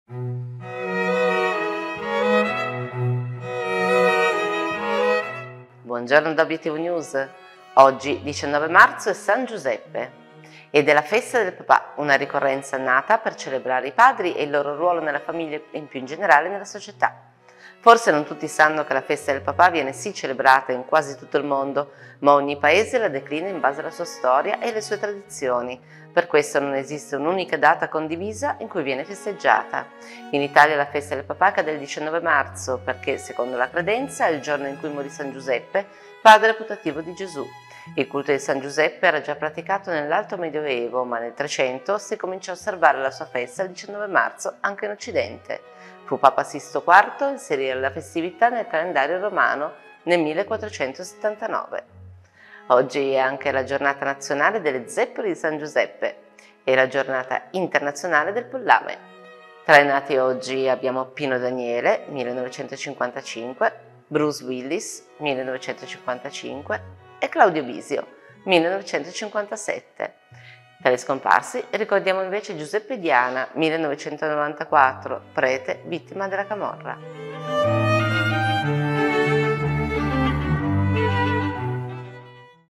OGGI È - 19 marzo San Giuseppe: Festa del papà
Oggi, 19 marzo, è San Giuseppe ed la Festa del Papà una ricorrenza nata per celebrare i padri e il loro ruolo nella famiglia e più in genrale nelle società..
Forse non tutti sanno che la festa del papà viene sì celebrata in quasi tutto il mondo, ma ogni Paese la declina in base alla sua storia e alle sue tradizioni. Per questo non esiste un’unica data condivisa in cui viene festeggiata. In Italia, la festa del papà cade il 19 marzo perché, secondo la credenza, è il giorno in cui morì San Giuseppe, padre putativo di Gesù.
Il culto di San Giuseppe era già praticato nell’Alto Medioevo, ma nel Trecento si cominciò a osservare la sua festa il 19 marzo, anche in Occidente. Fu papa Sisto IV a inserire la festività nel calendario romano, nel 1479.
Oggi è anche la Giornata Nazionale delle zeppole di San Giuseppe e la Giornata Internazionale del pollame.
Tra i nati oggi abbiamo Pino Daniele (1955);  Bruce Willis (1955); e Claudio Bisio (1957).
Tra gli scomparsi ricordiamo Giuseppe Diana (1994), prete vittima della Camorra.